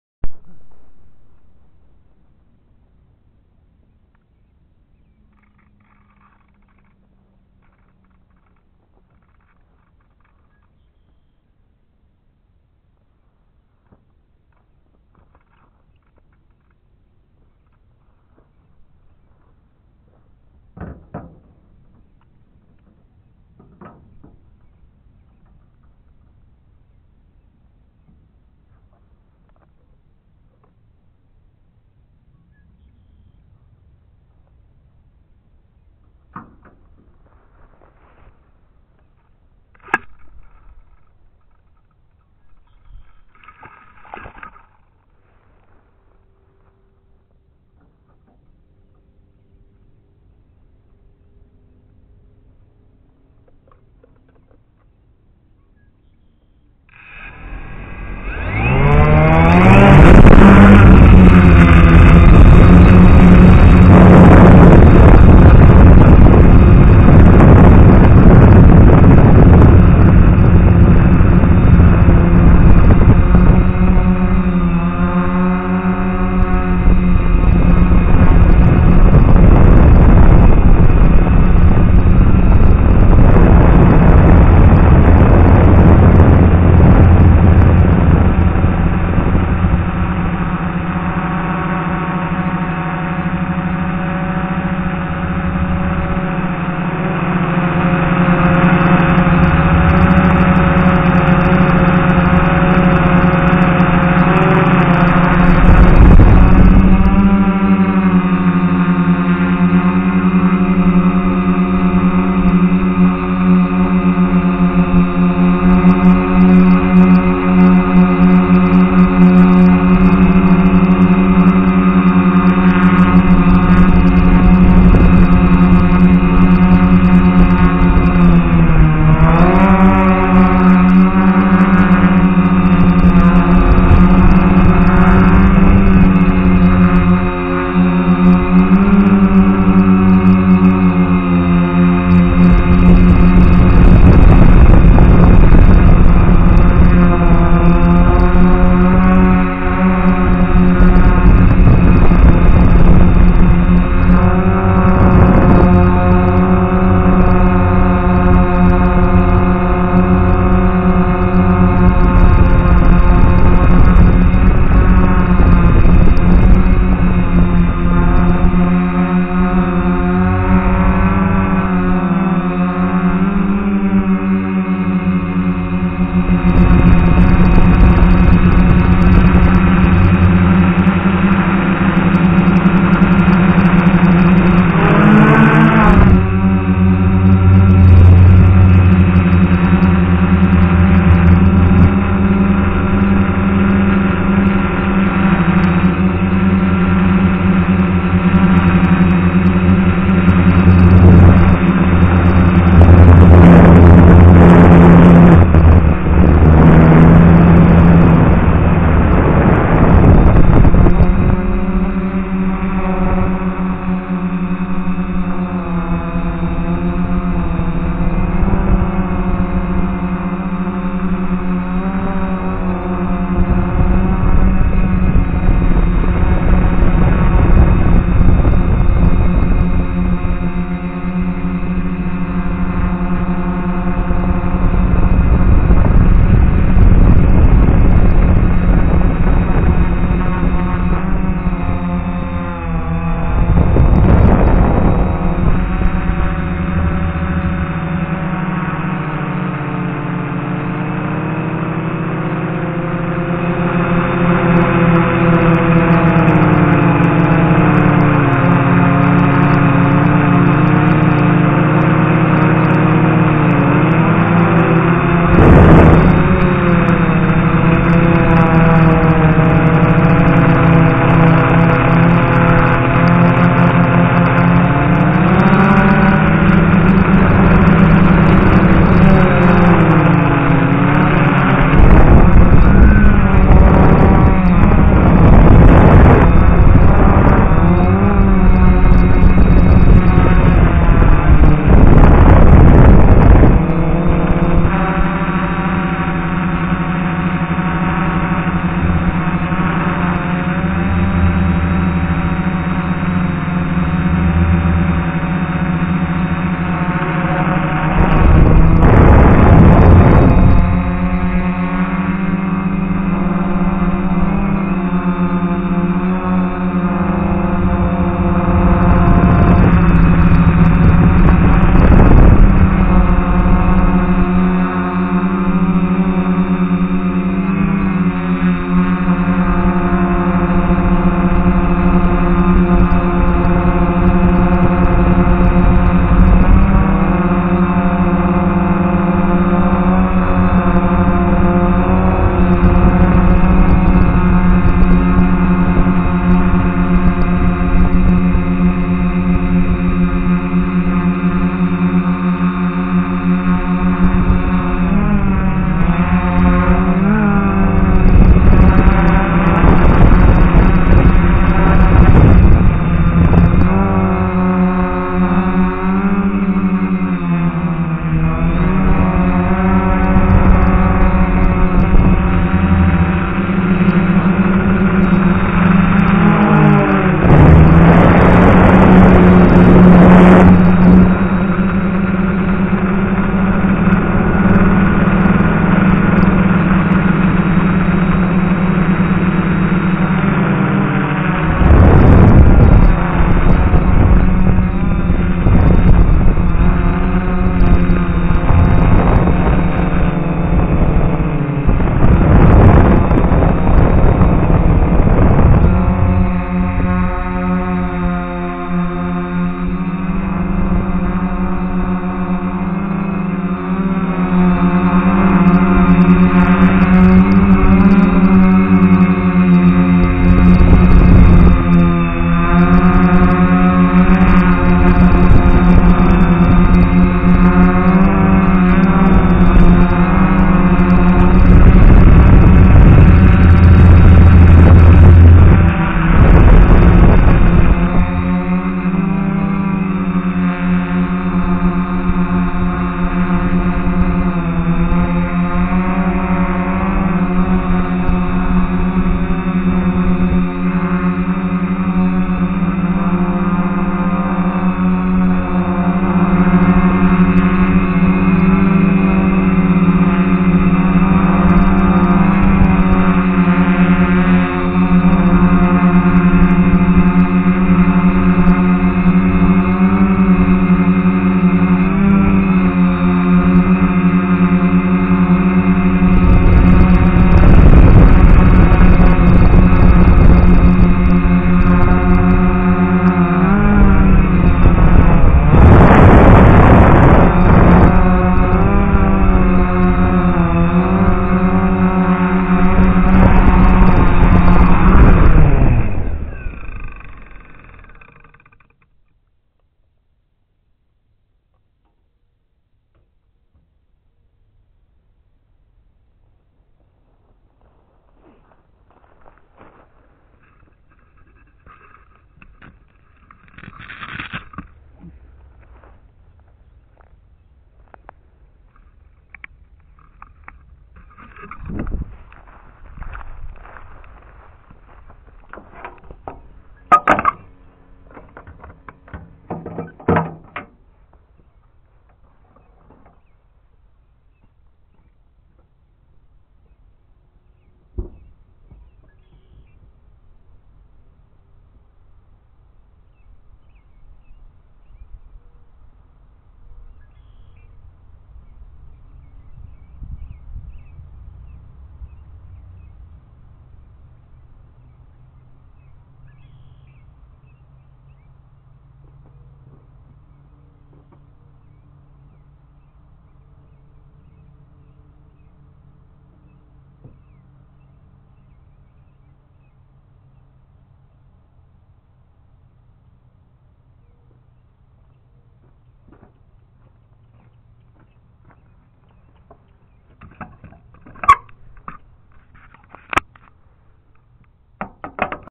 FPV - Exploring # 5 "Exploring old tool's"
I useing a dji -f450 to see the hill side  of a old coal mine and look at some old mining tools . This flight is done by fpv flying .By  black diamond wa